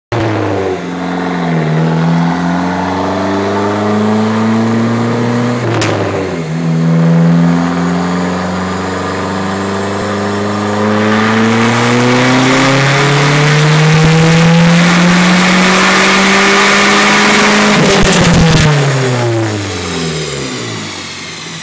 SRT-4 2003 Dyno @ NRG Tech w/ NYSRTCrew Part2
2003 only mods are CHQ 3' Catless Downpipe & intake!!! 219WHP 231TQ